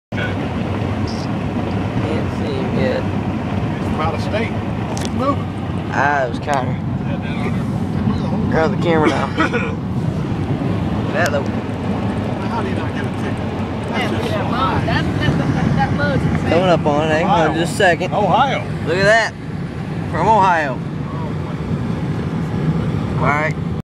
OVER SIZED LOAD!!!!!!!
A pick up truck that is overlaoded